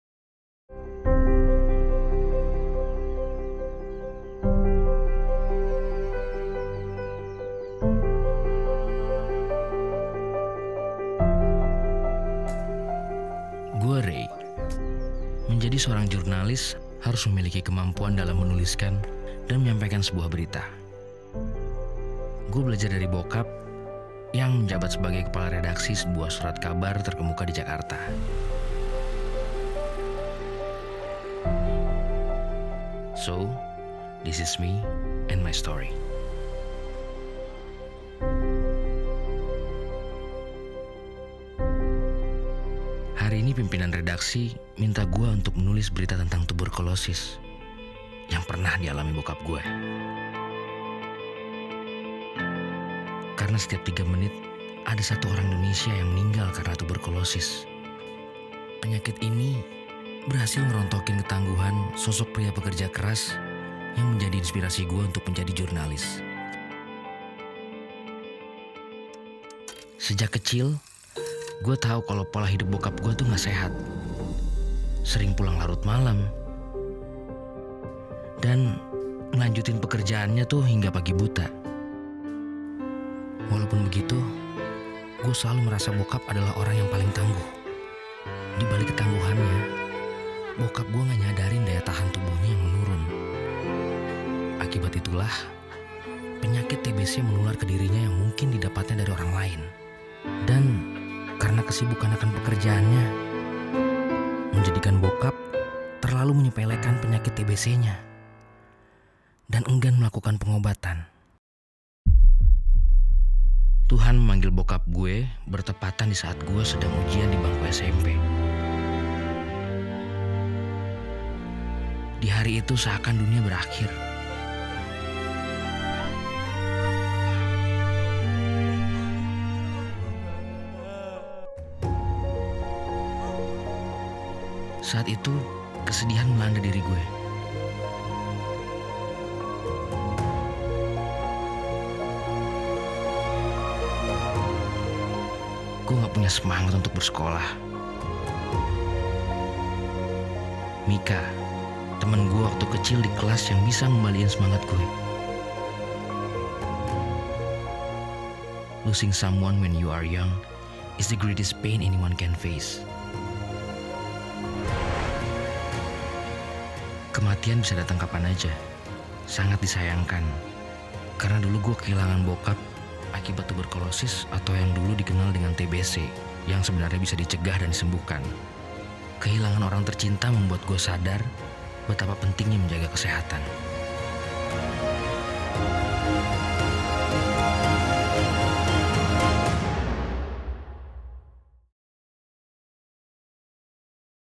This is Me and My Story - Eps. 1 "TBC Merenggut Nyawa Ayahku" - Ayotosstb
Sebuah cerita pengalaman seorang jurnalis bernama Rey. Rey terinspirasi dari sang Ayah yang menjadi seoarng pimpinan redaksi di sebuah surat kabar. Karena kesibukan sang ayah dalam melakukan pekerjaan dan menyepelekan penyakit tuberkulosis akhirnya sang ayah meninggal disaat Rey masih duduk di bangku SMP. Disaat Rey merasa kesedihan datanglah sosok Mika, sahabat Rey di SMP yang menyemangatinya untuk tetap bersekolah dan mengejar mimpinya untuk menjadi seorang jurnalis.